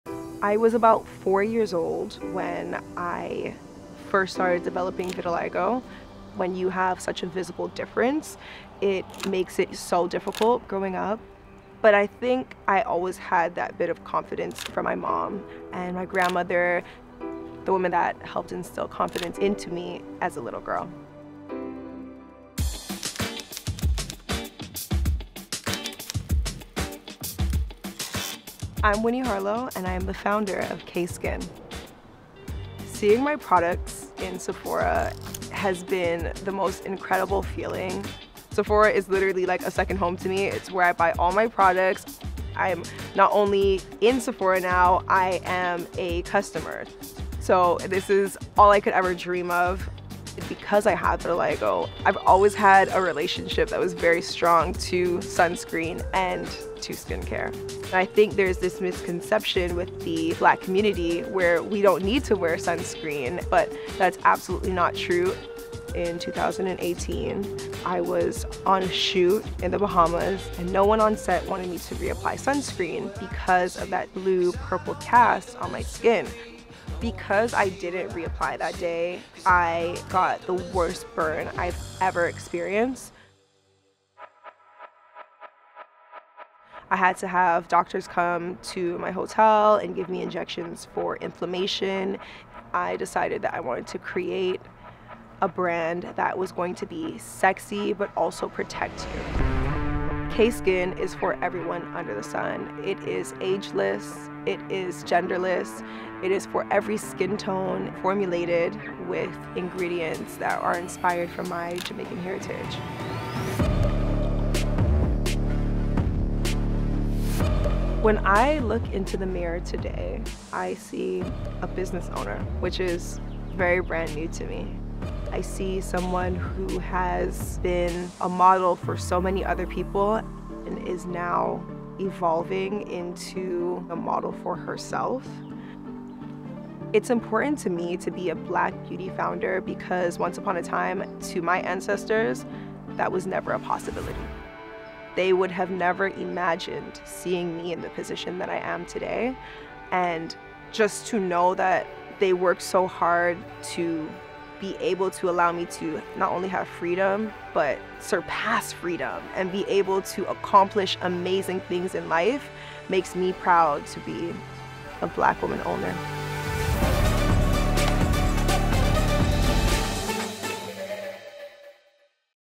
Winnie Harlow's Journey to Becoming a Black Beauty Founder | Sephora
“It’s important to me to be a Black beauty founder because once upon a time, to my ancestors that was never a possibility.”

We’re honored Winnie (@winnieharlow), founder of @cayskin, chose to share her story with us. We’re excited to watch you lead the way for the next generation of Black beauty founders ❤️